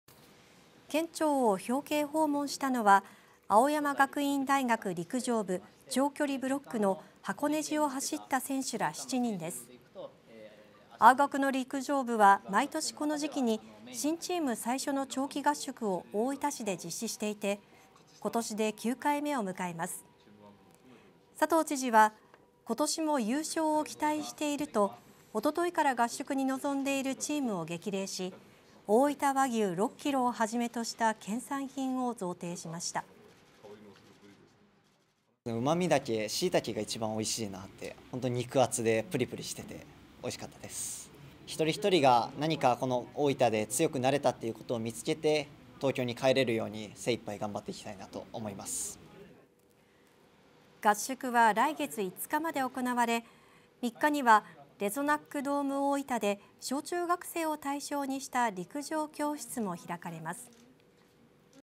青山学院大学陸上部が県庁訪問 今季の活躍誓う 大分で強化合宿
1月の箱根駅伝で優勝し、大分市で強化合宿を開始した青山学院大学の陸上部が県庁を訪れ、今シーズンの活躍を誓いました。

 / obs_hodo